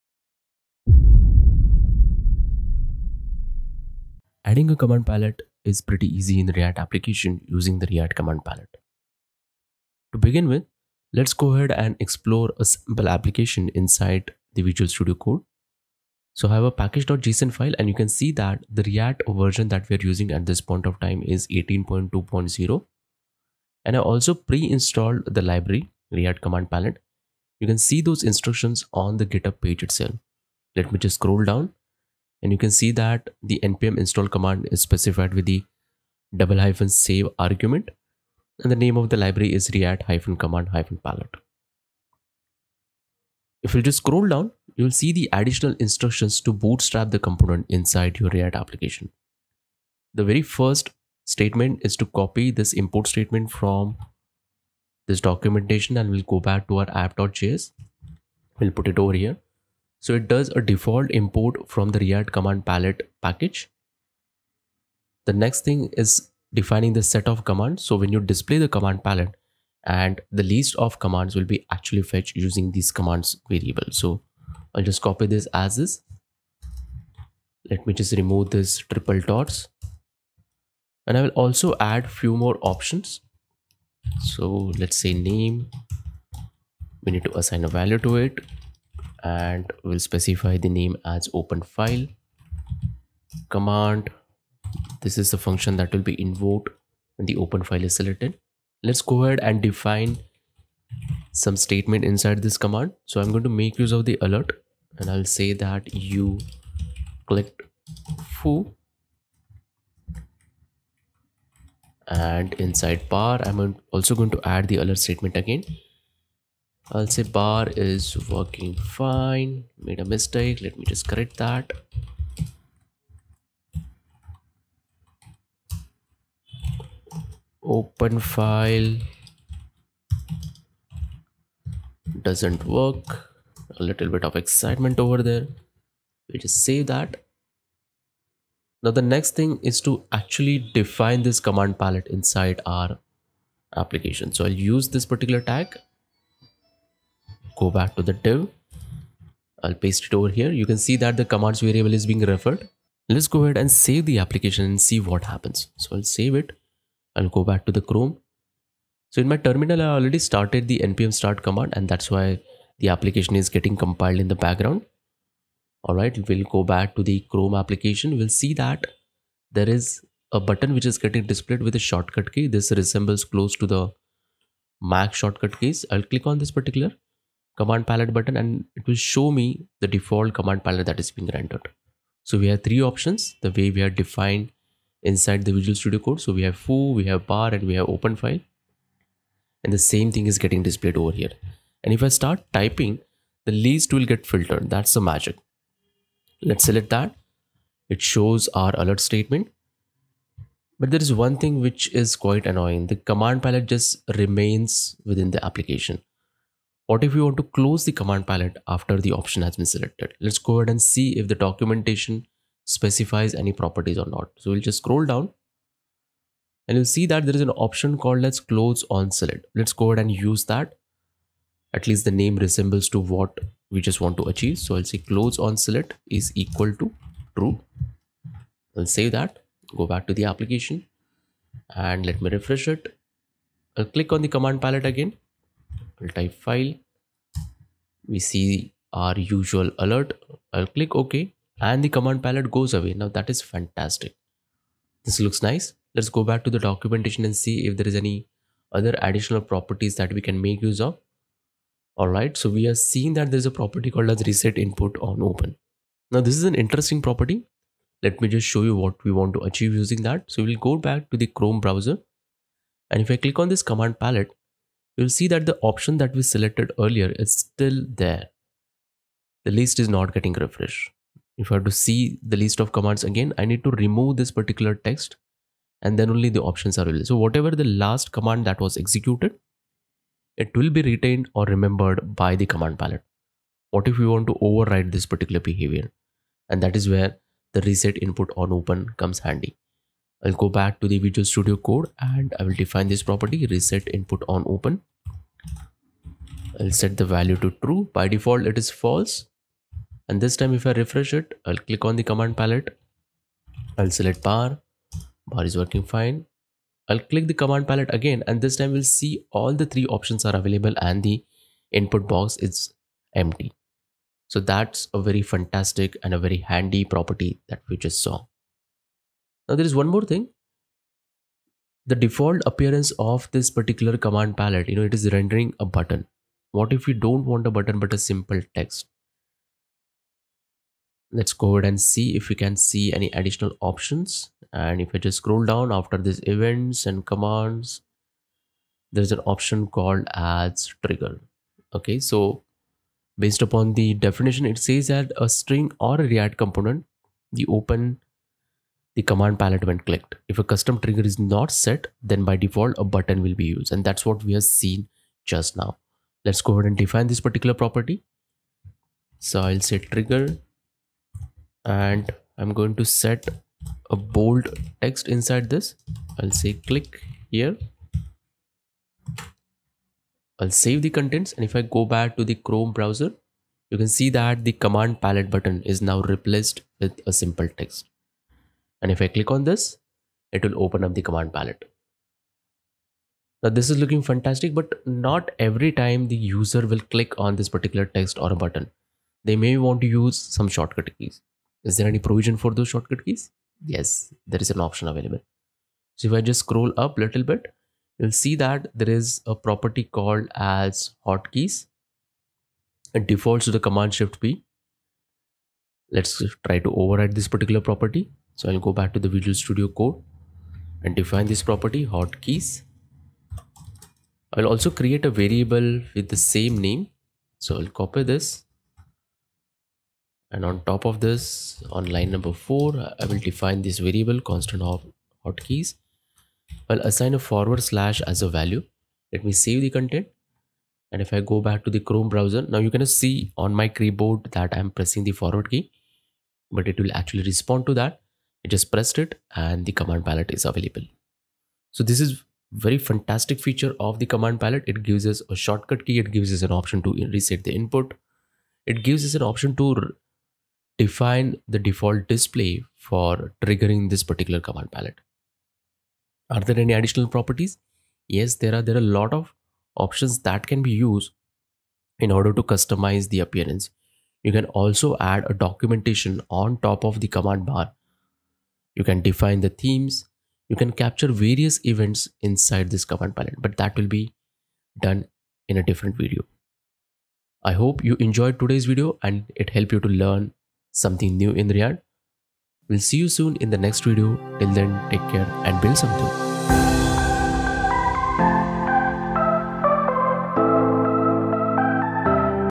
Command Palette in React 18
Learn how to add a command palette in React application.